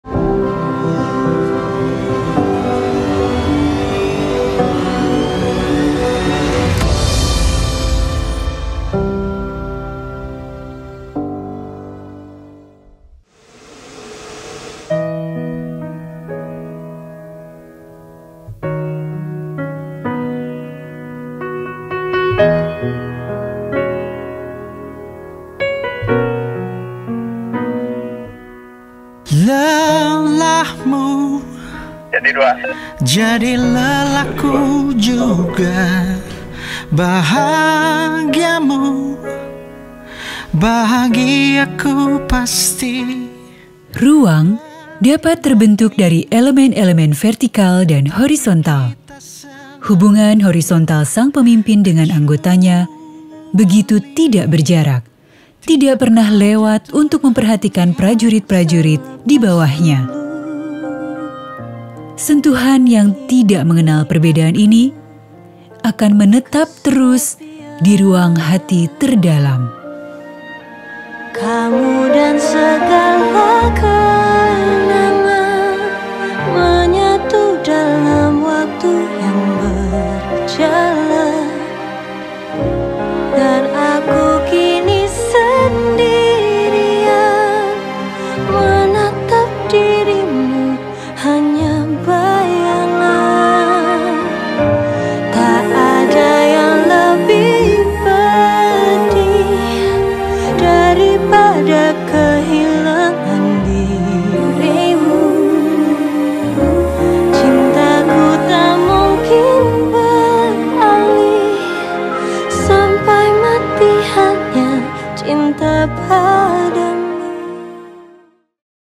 Part 12: Hubungan Tak Berjarak, Antara Atasan-Bawahan | Ruang Kita
Selayang pandang interaksi sosial-budaya Jenderal TNI Andika Perkasa dan Ibu Hetty bersama masyarakat yang ditemui dalam kegiatan sehari-hari, serta cerita kegiatan Ibu Hetty sehari-hari.